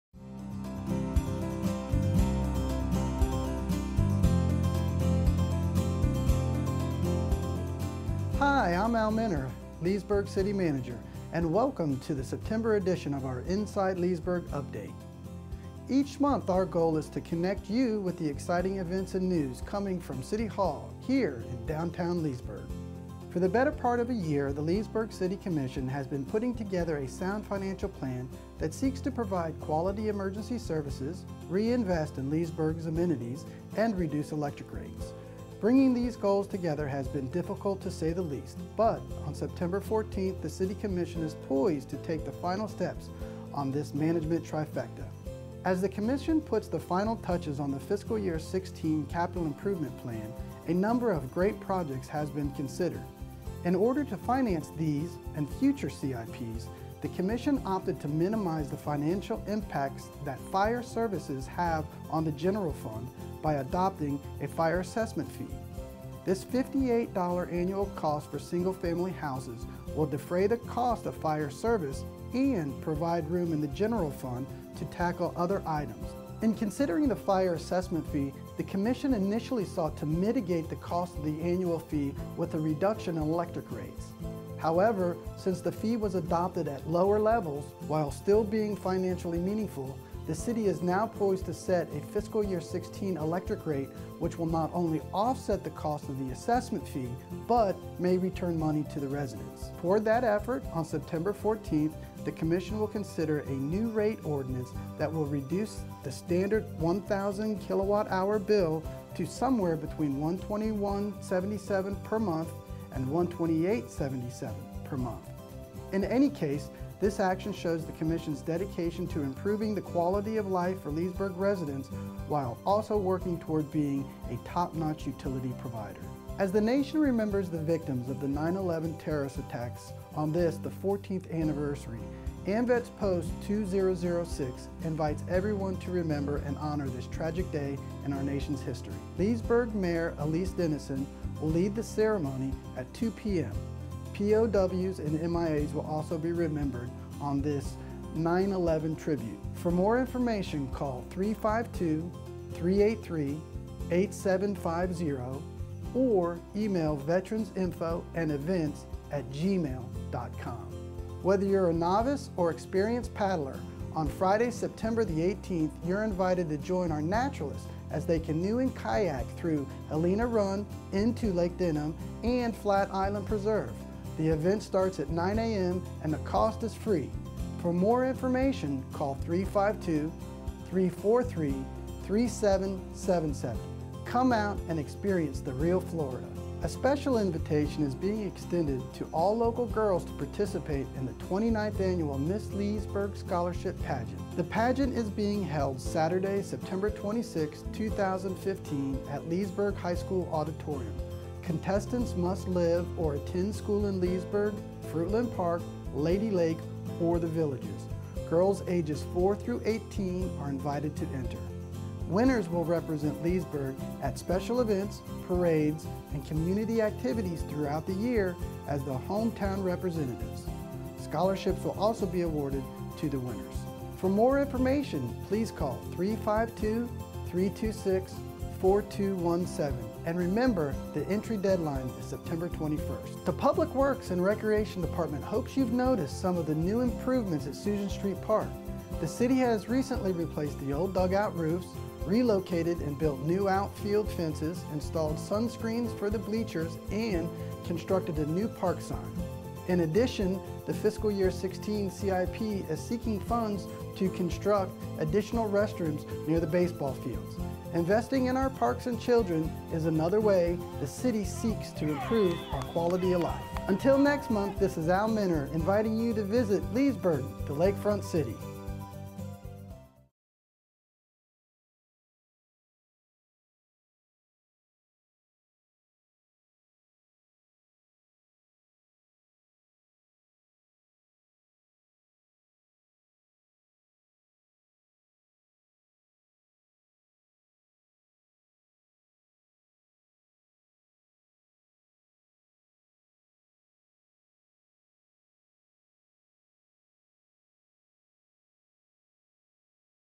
Inside Leesburg Update
Here is your latest news and events for September 2015 from City Manager Al Minner